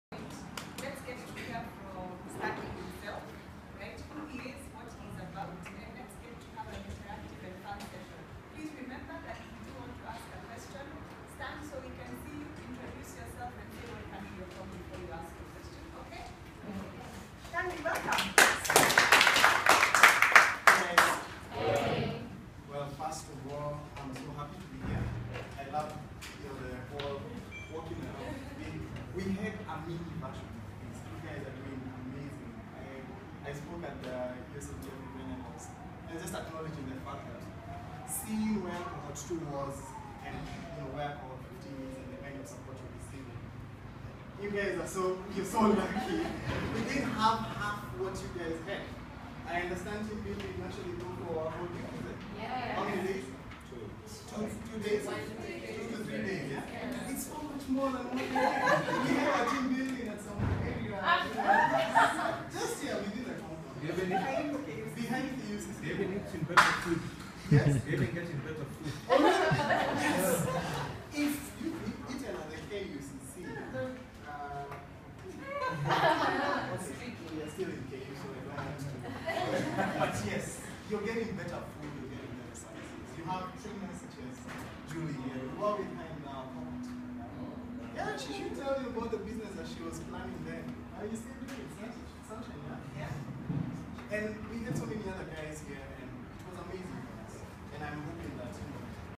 Six Degrees Consultancy CEO, Stanley Mburu Gitau, addressing YALI RLC EA Cohort 15
On 3rd March 2017, Six Degrees Consultancy, CEO Stanley Mburu Gitau, honored an invite he received from YALI RLC EA to have a conversation with YALI Cohort 15 pursuing Business and Entrepreneurship Track aimed at exploring his Entrepreneurial Journey, The USADF Grant Award and impact the YALI RLC program had contributed towards starting Six Degrees Consultancy